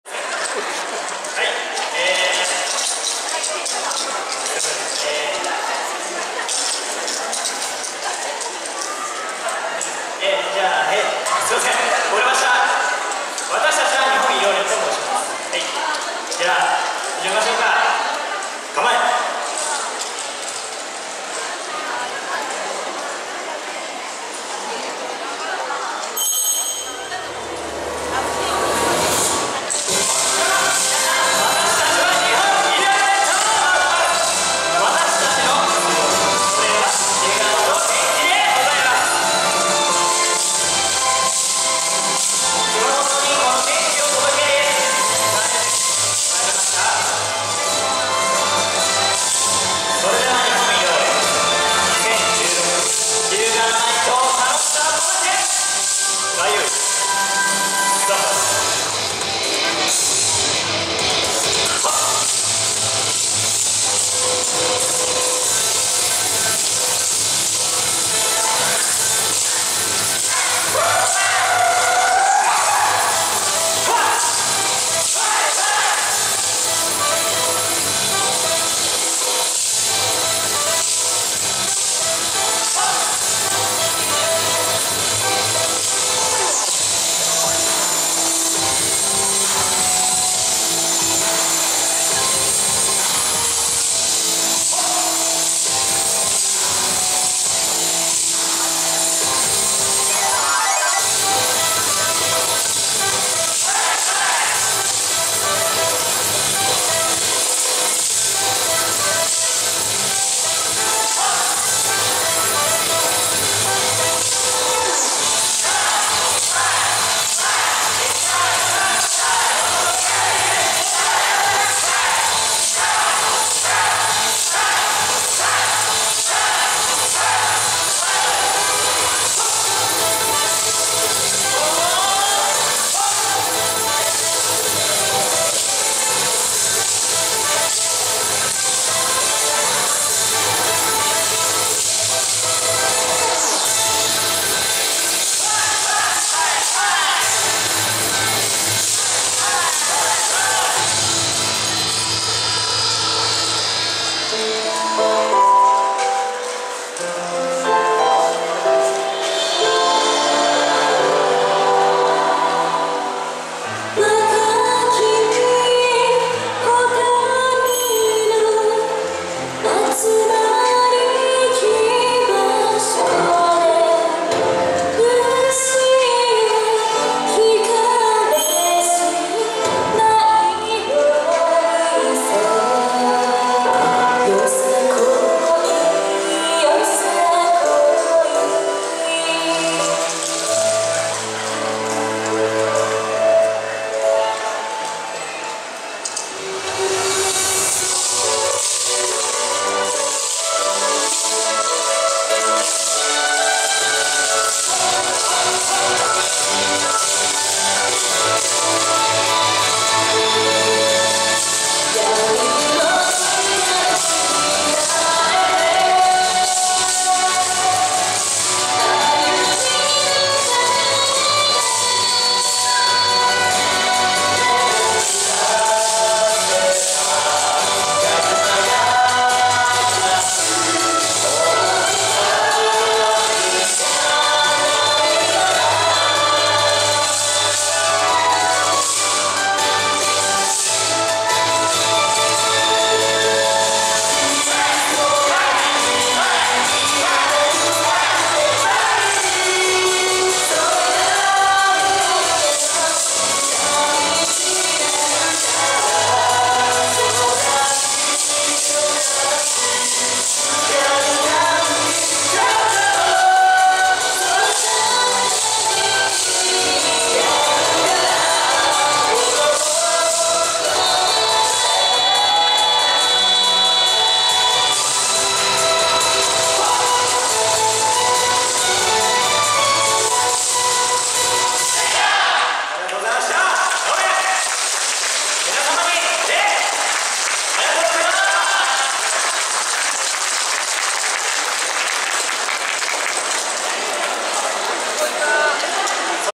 日本医療連 （1st）／ワカバウォーク熊本地震チャリティーよさこい
2016/07/03
ワカバウォーク 熊本地震チャリティーよさこい
【会場】東武東上線若葉駅東口ワカバウォークイベント広場（鶴ヶ島市）
【主催】地元及び近在チーム有志による開催